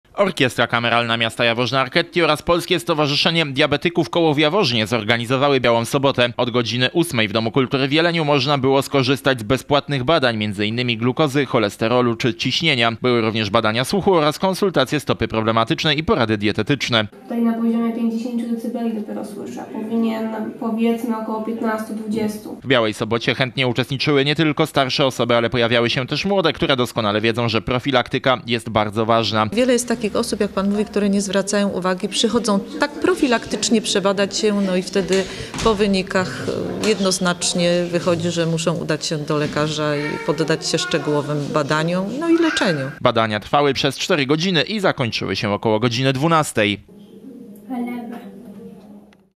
Biała Sobota w Domu Kultury w Jeleniu
Orkiestra Kameralna Miasta Jaworzna Archetti oraz Polskie Stowarzyszenie Diabetyków Koło w Jaworznie zorganizowały „Białą Sobotę”. Od godziny 8 w Domu Kultury w Jeleniu można było skorzystać z bezpłatnych badań, min. glukozy, cholesterolu, czy ciśnienia. Były również badania słuchu oraz konsultacje stopy problematycznej i porady dietetyczne.

W „Białej Sobocie” chętnie uczestniczyły nie tylko starsze osoby, ale pojawiały się też młode, które doskonale wiedzą, że profilaktyka jest bardzo ważna.

Badania trwały przez cztery godziny i zakończyły się około godziny 12.